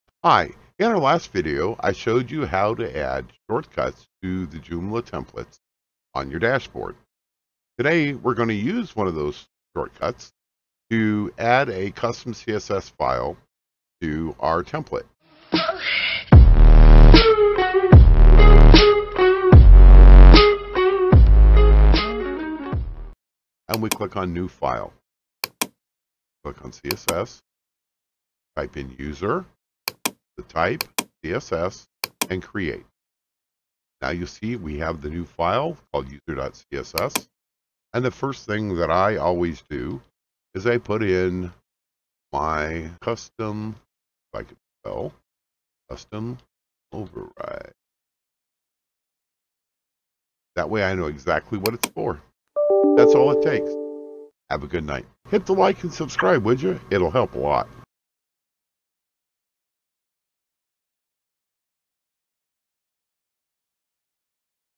How to create custom.css file in Joomla 4
Joomla 4 no longer uses "custom.css" for custom overrides, instead it uses "user.css".
Today I'll show you how to quickly create that file.